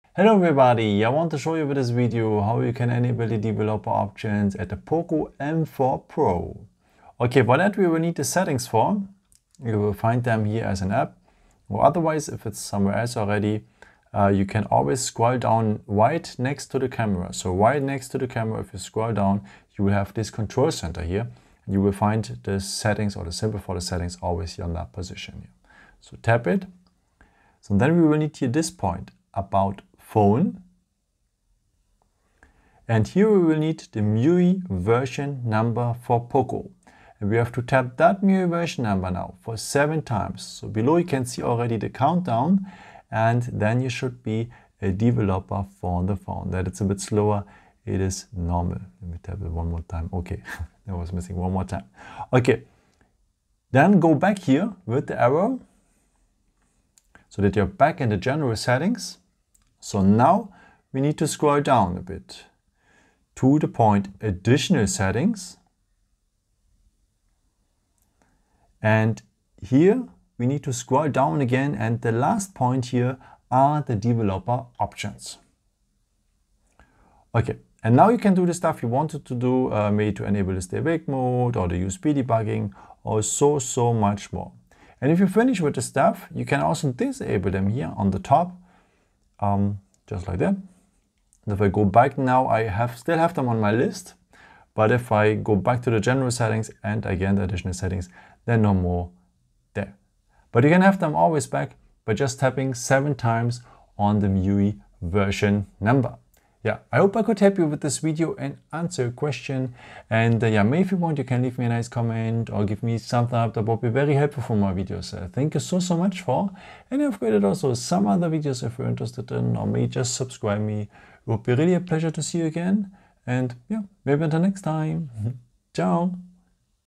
Poco M4 Pro (5G): How to enable the Developer Options? for USB Debugging etc.
With this video, I want to show you, how you can activate the developer options at the Xiaomi Poco M4 Pro 5G.
I will show you also, how you can disable it after.At the developer's mode you can enable the USB-Debugging to transfer data for example.